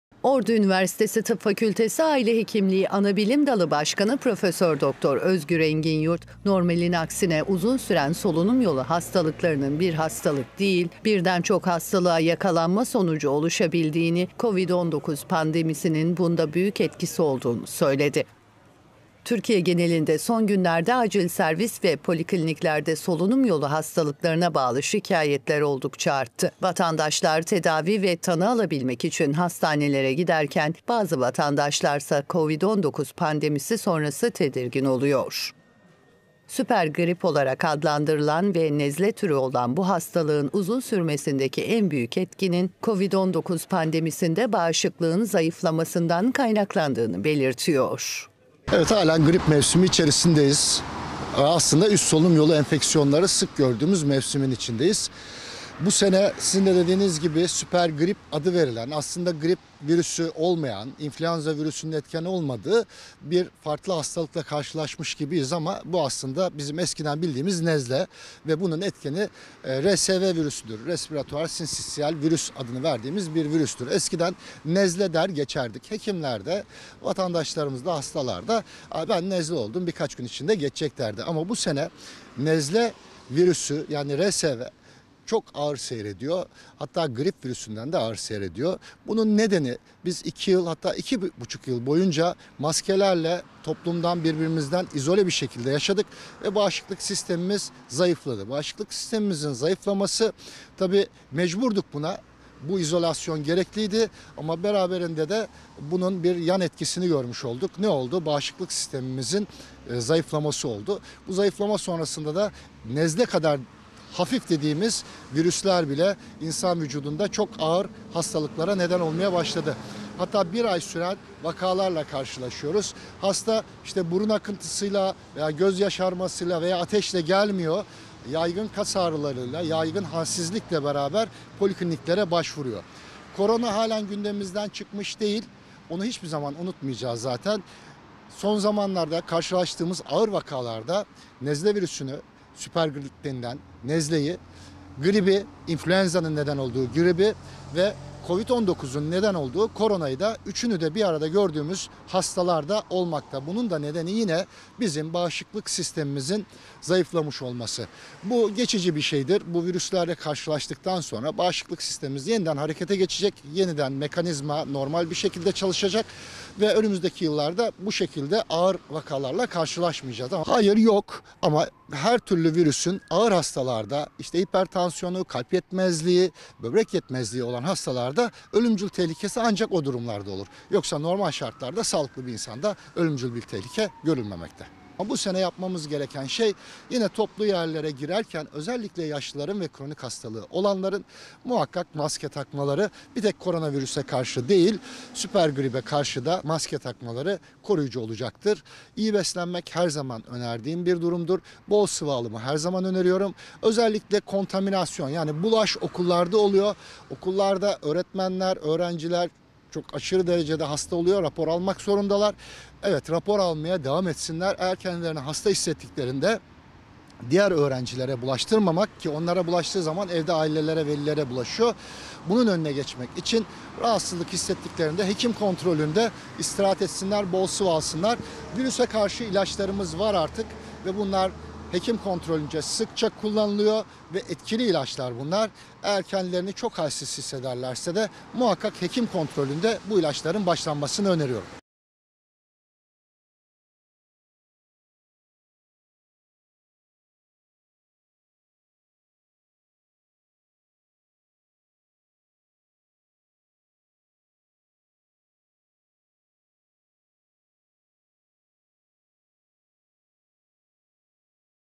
Süper Grip Neden Uzun Sürüyor? Uzman İsim Sebebini Açıkladı
2023

Ordu Üniversitesi Tıp Fakültesi Aile Hekimliği Anabilim Dalı Başkanı Prof. Dr. Özgür Enginyurt, normalin aksine uzun süren solunum yolu hastalıklarının bir hastalık değil, birden çok hastalığa yakalanma sonucu oluşabildiğini, Kovid-19 pandemisinin bunda büyük etkisi olduğunu söyledi.